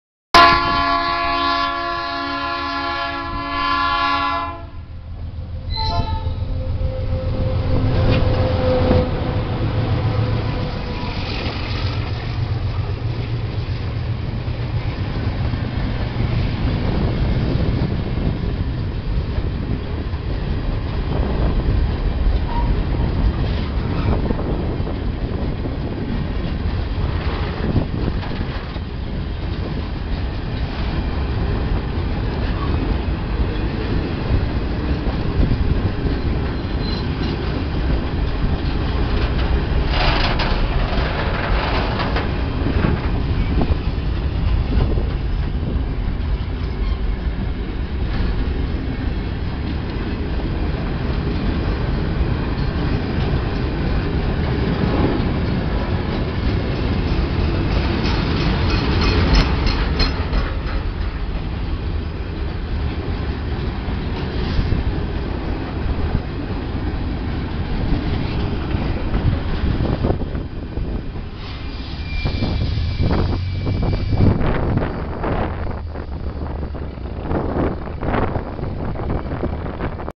Tren cargado de NCA con la ALCo RSD16 8231
En la fresca y lluviosa mañana de hoy la ALCo RSD16 8231 mas un corto tren compuesto por 38 tolvas graneras cargadas procedentes de Tancacha va entrando a la Ciudad de Marcos Juárez con rumbo a Rosario. Marcos Juárez. Córdoba. Argentina. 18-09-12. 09:10 a.m.